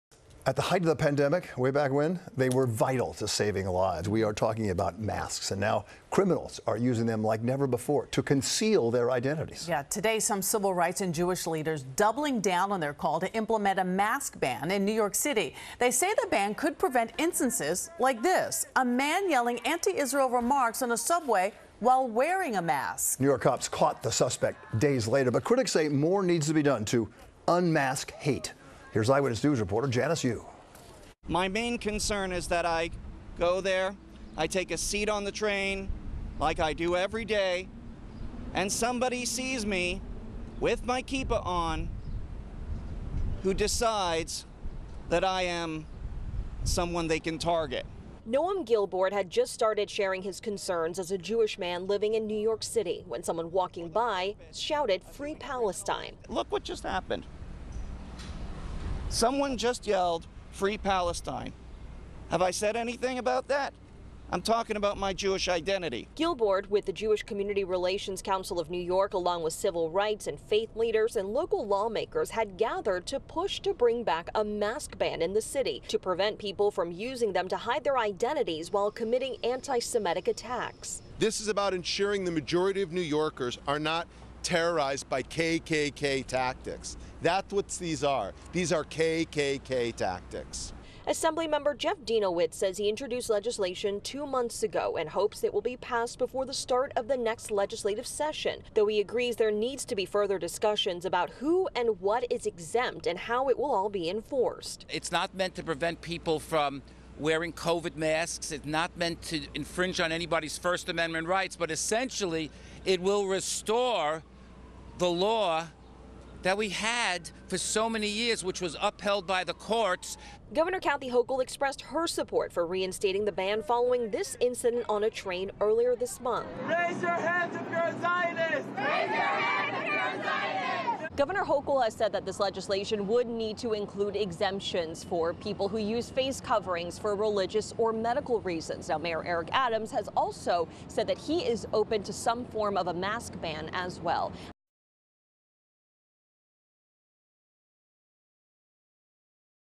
Civil rights leaders rally to ban protesters from wearing masks
Janice Yu has more from Columbia University.